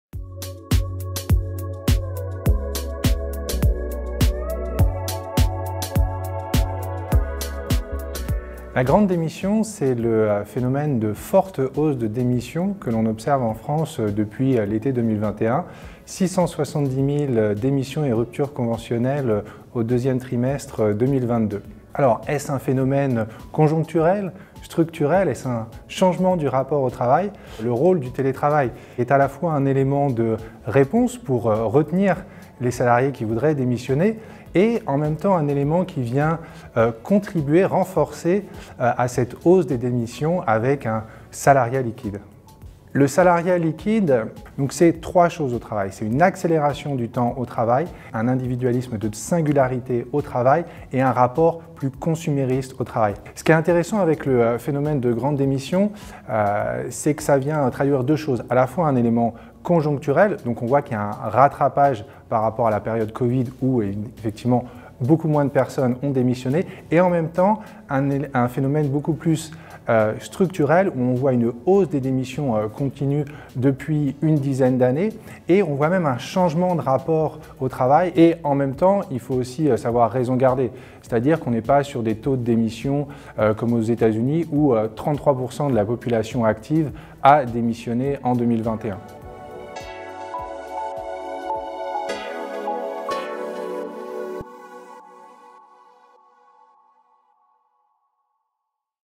La grand démission ? Interview de Vincent Meyer
Vincent Meyer, enseignant-chercheur@emnormandieMétis Lab, explique en quoi consiste le phénomène dit de « grande démission » qui défraie la chronique depuis la crise sanitaire. Le télétravail peut-il être un levier pour attirer/conserver des ressources humaines dans les entreprises ?

Cette interview a été réalisée dans le cadre de la sortie du n° 181 des Cahiers de L'Institut « À distance - La révolution du télétravail », un numéro coédité avec les @editionspuf.